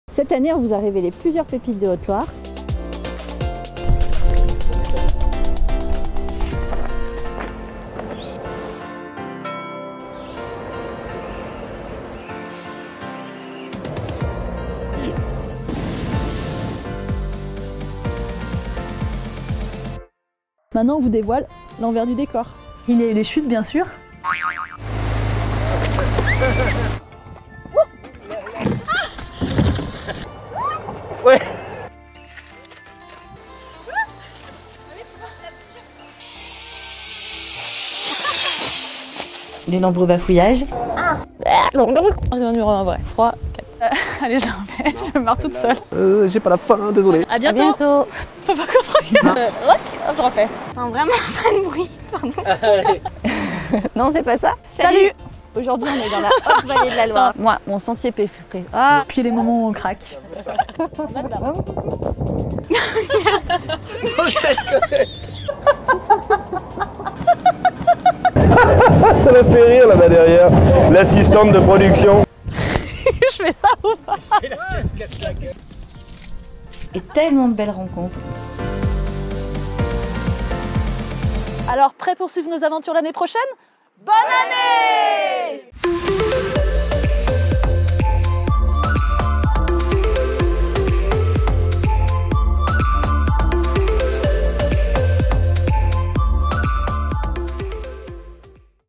Pépites et bêtises pour commencer l’année 2019
En 2018, on vous a embarqué  dans nos aventures à la recherche de  nos plus belles pépites.

Pour bien commencer l’année, on vous a concocté un best of de nos plus belles images  et aussi de belles surprises, histoire de vous révéler l’envers du décor !

Prêts à  vivre d’autres expériences avec nous en 2019?

Belle  année  avec  de belles pépites sur votre chemin !